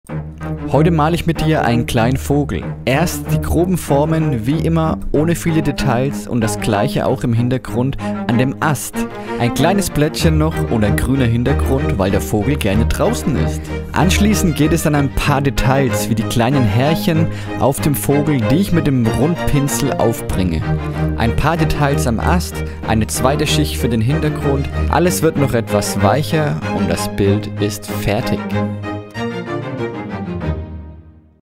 Vogel malen mit Öldarben
In diesem Video male ich einen Vogel mit Ölfarben.
Du siehst verschiedene Ölmalerei Techniken und lernst, wie du als Anfänger das Ölmalen lernst.

So wir das malen mit Ölfarben ganz einfach.